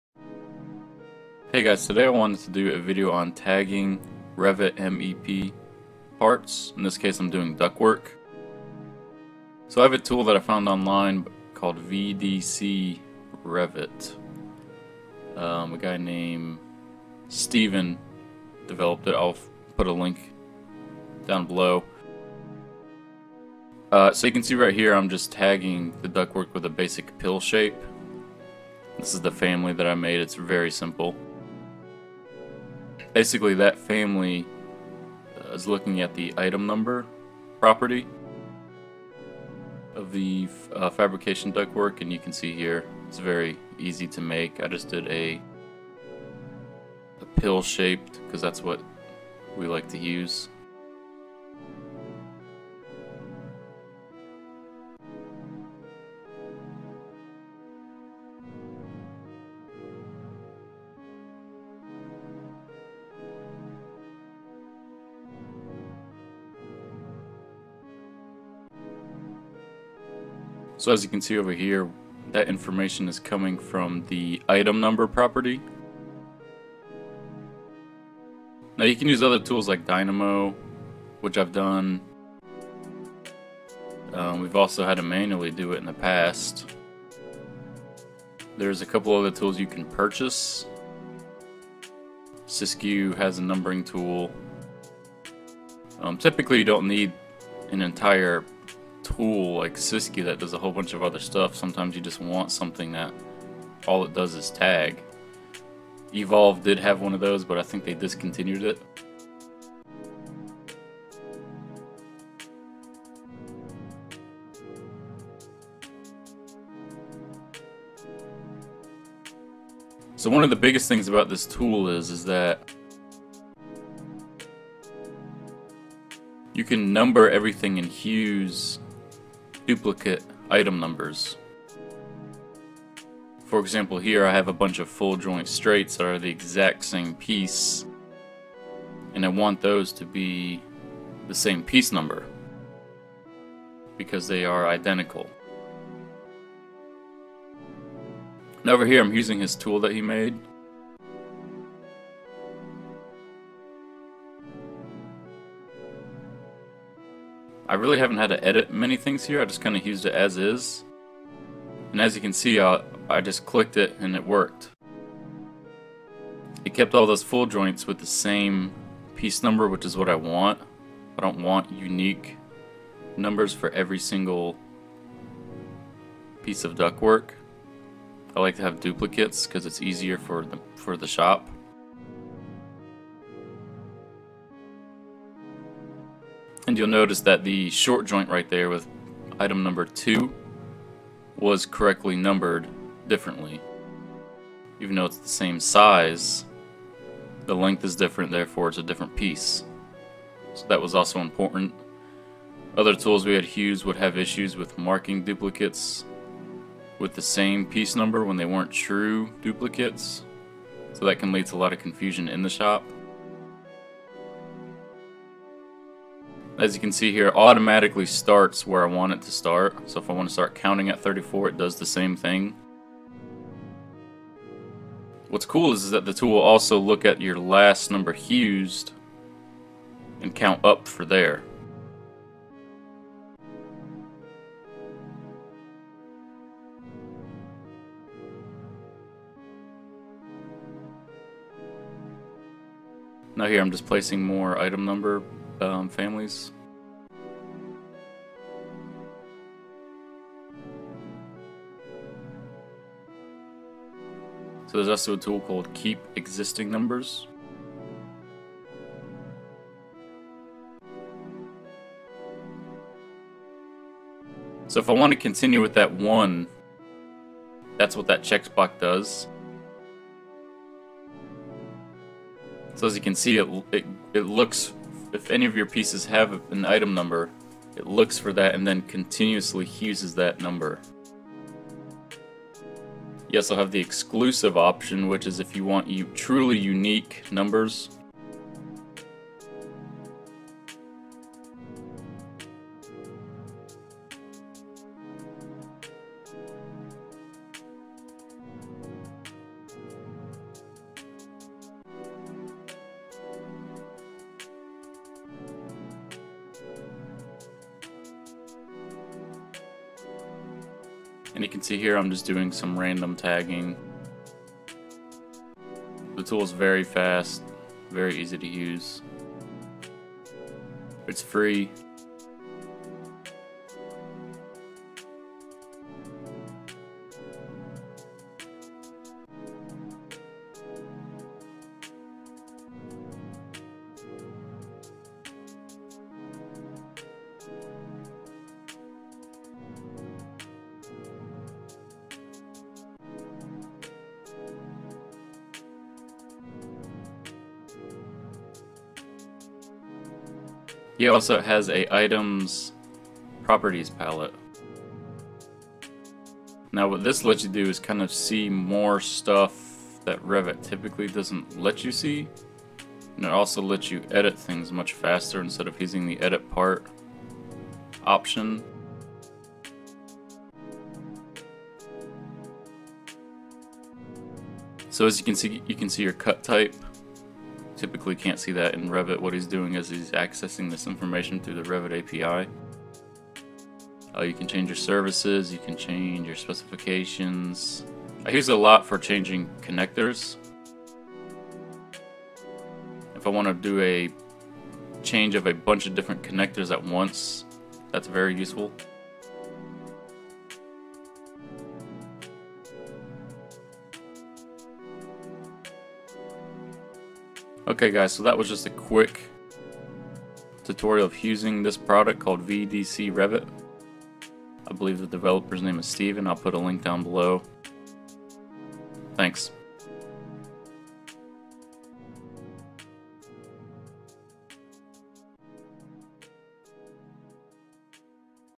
Tagging Revit MEP Parts Automatically (Free tool)
This is the link for the free tool. Currently it only is available for Revit 2018, 2019, and 2020. I have been using it for some time now and it pretty much works perfectly.